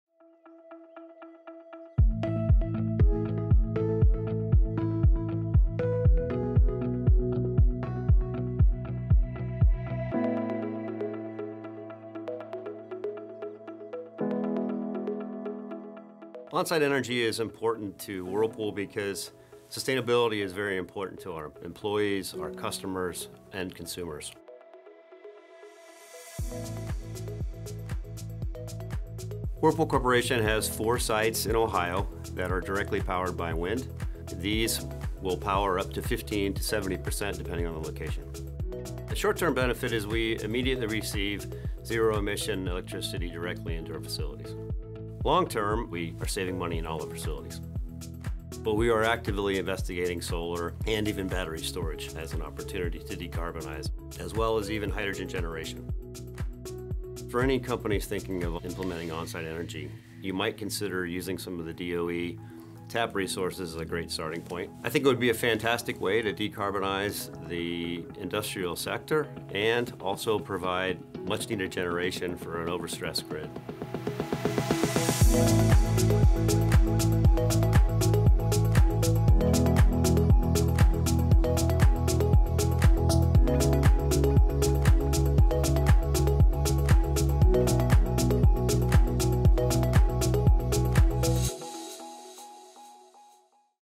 U.S. Department of Energy Goes Onsite to Reduce Industrial Emissions
The Department of Energy’s Onsite Energy Program helps industrial organizations and other large energy users adopt onsite clean energy technologies to reduce emissions, save money, and gain greater control over how energy is used. Watch the video to see how Whirlpool has curbed emissions and operating costs by leveraging onsite energy at its facilities in Ohio.